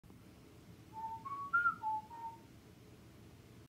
Guess the whistle tune of this commercial #1
Hint: they have ice cream cakes, ice cream treats, you could sometimes get an ice cream cone, ice cream sundae, banana split or you can order just other food